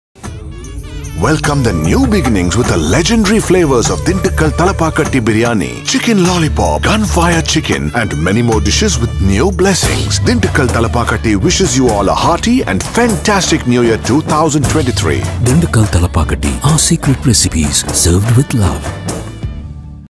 Happy New Year 2023 | Dindigul Thalappakatti
It's time to sit down with your loved ones and relish the taste of our 65-year-old secret recipes and welcome new beginnings!

Dindigul Thalappakatti wishes you a very Happy New Year!